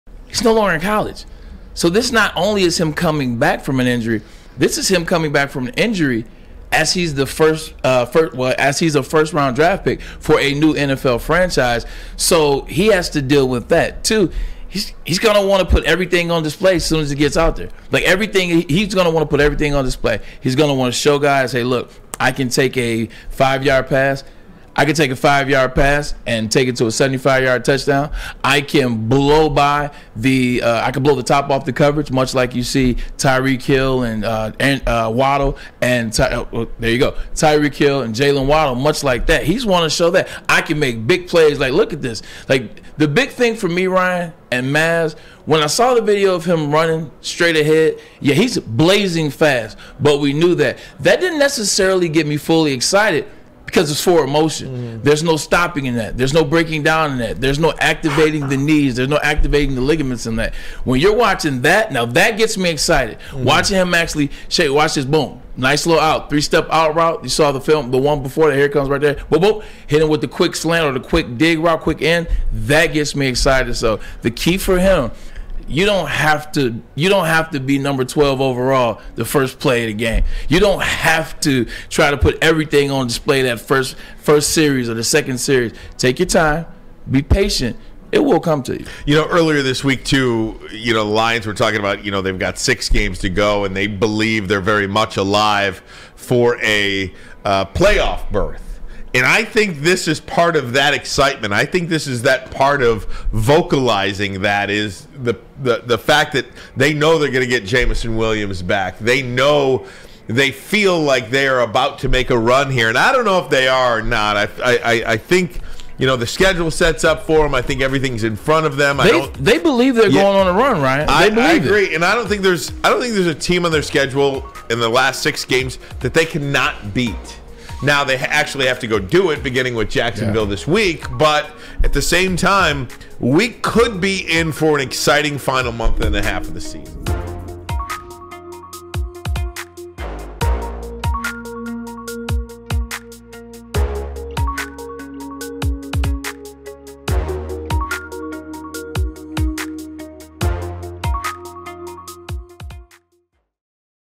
Will Jameson Williams Play on Sunday for the Detroit Lions?
With Jameson Williams back to full practice, will Dan Campbell and the Detroit Lions decide to integrate him into their game plan versus the Jacksonville Jaguars?